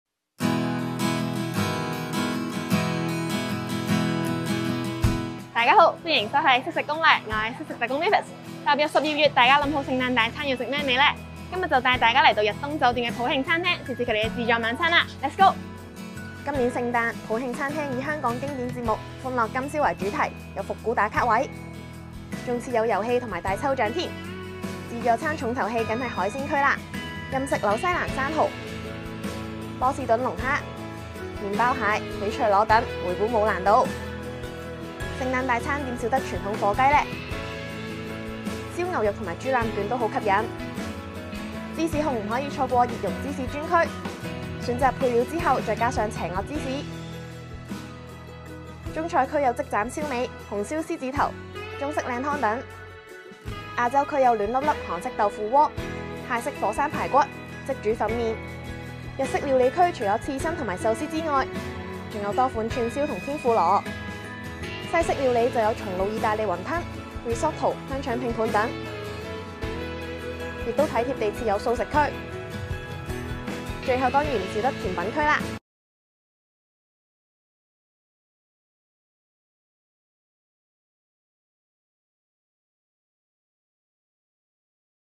【識食攻略】聖誕大餐食乜好！逸東酒店普慶餐廳聖誕自助餐 3小時任食生蠔／龍蝦／刺身／火雞／熱熔芝士專區
踏入12月，香港逸東酒店普慶餐廳推出聖誕自助餐，12月份午市及晚市自助餐都會換上「普慶今宵」主題。有多款海鮮、熱食和甜品等大家食足三個鐘，更特別設置遊戲攤位，可以玩《歡樂今宵》經典遊戲贏取豐富獎品！